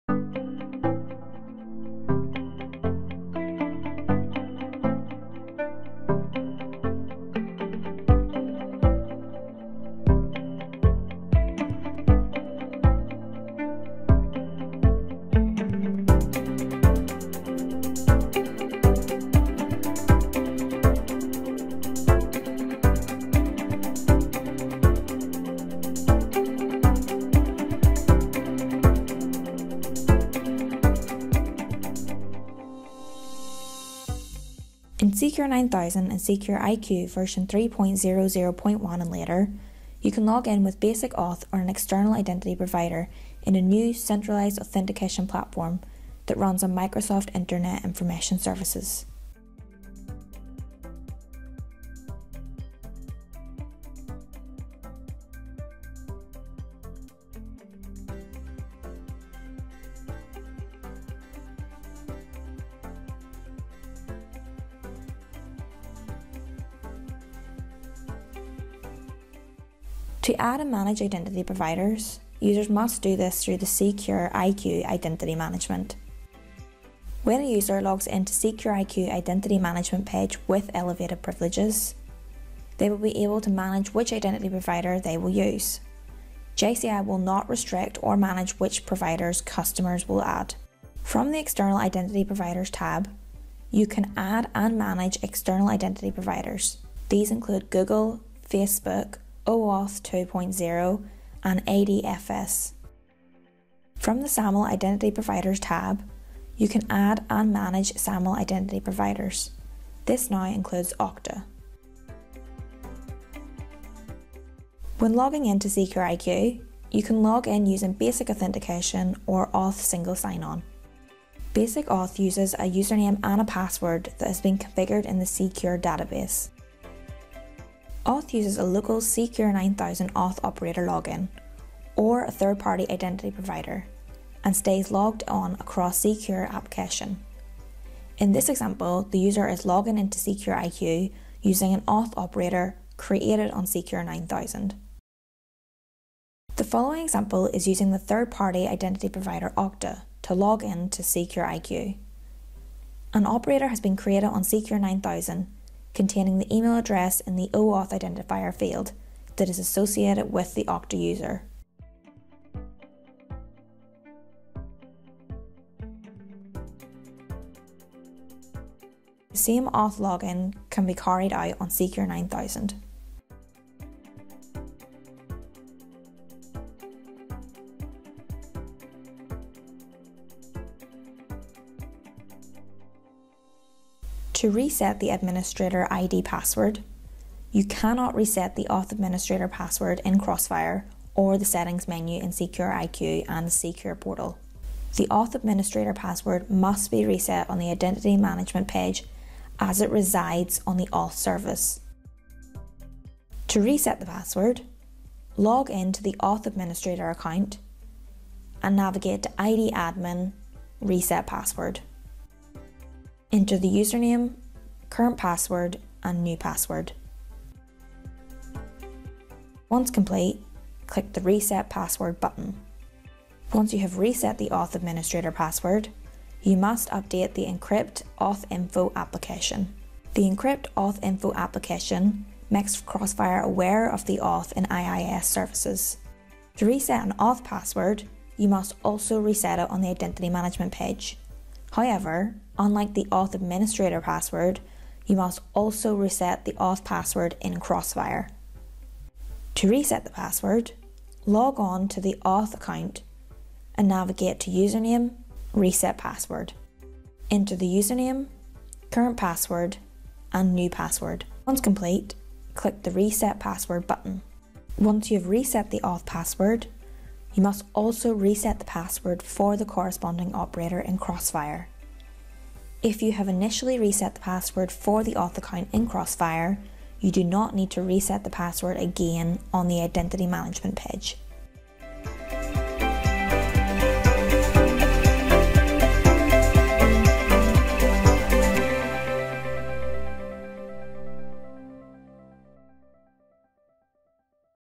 Authentication Through IIS
C•CURE 9000, C•CURE IQ Security Client and C•CURE Portal allows you to login with basic auth or an external identity provider in a new centralised authentication platform that runs on Microsoft Internet Information Services (IIS).

00:00 Introduction
00:36 Authentication Standards
01:11 Identity Management
01:55 C•CURE IQ Basic Authentication Login
02:25 C•CURE IQ Identity Provider (Okta) Login
02:47 C•CURE 9000 Basic Authentication Login
03:05 Reset Auth Admin (idadmin) Password
03:55 Reset Auth Password